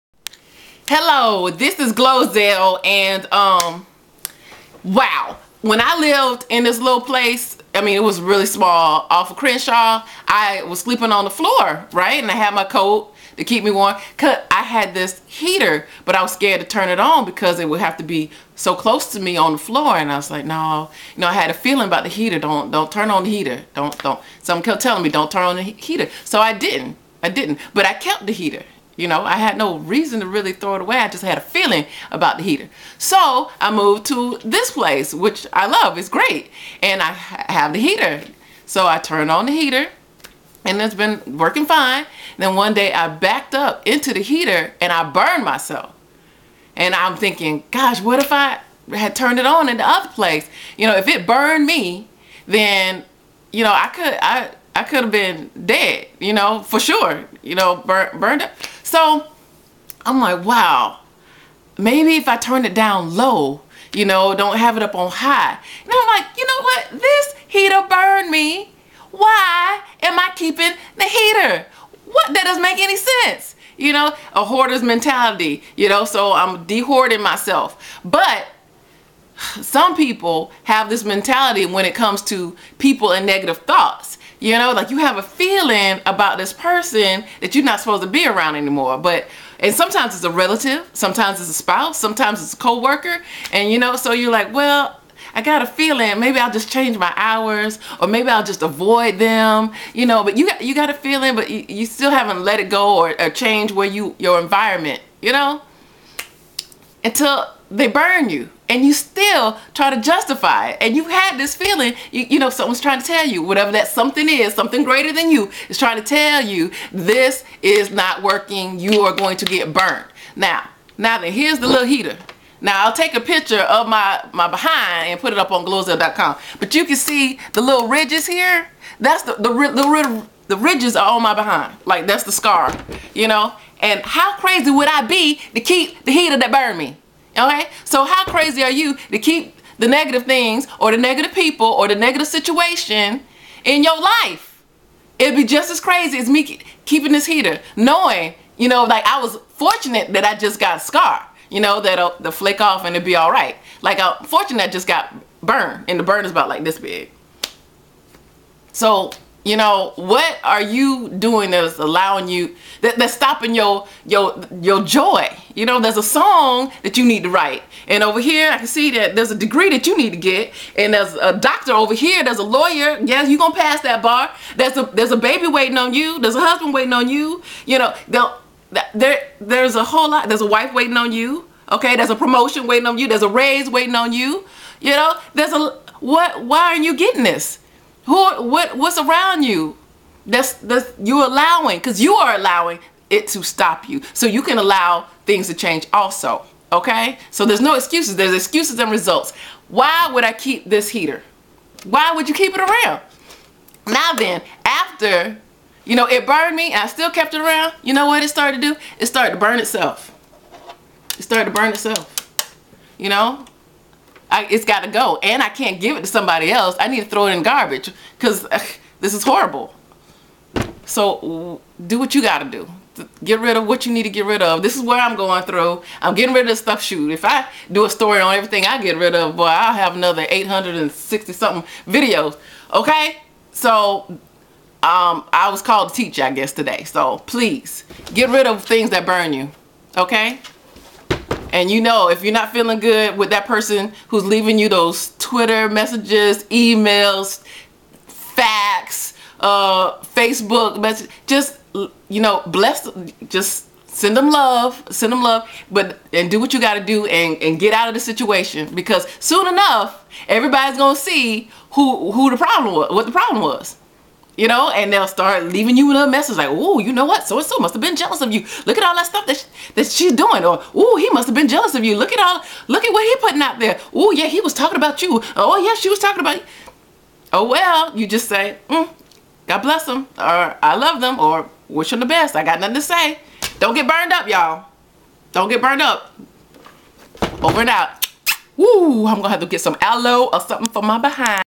How many times do you need to get burned ... By GloZell
You have to rid of toxic things... and people.